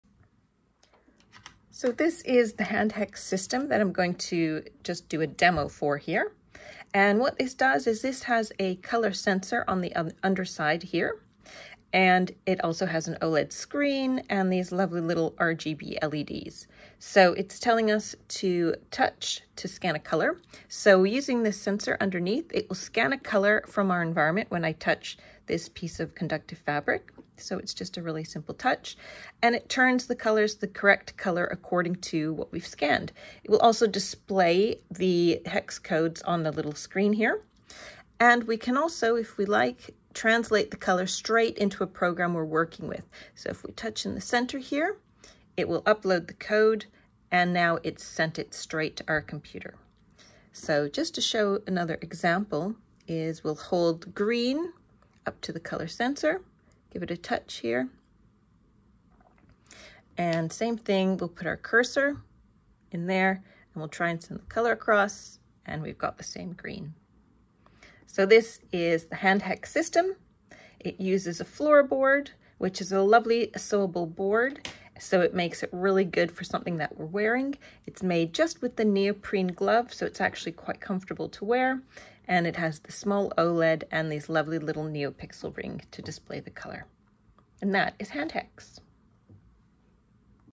The FUTURE is here - send HEX codes to your computer!
This is a demo of HandHEX, a wearable prototype that lets you scan any color in your environment—like something on your desk—and instantly view its HEX code on a small OLED screen.

Built using an ESP32 microcontroller, the latest version of HandHEX includes two touch buttons:
 • One button activates the color scan.
 • The second sends the HEX code directly to your computer, ready for use in your design or coding projects.

Whether you’re a maker, coder, or wearable tech enthusiast, HandHEX is a fun and functional tool for exploring color and integrating it into digital design workflows.

Tools & Tech Used:
 • ESP32 microcontroller
 • OLED display
 • Color sensor
 • Touch input
 • Arduino / PlatformIO

✔️ Created by: Tinker Tailor — creative tech kits and tools for wearables and prototyping.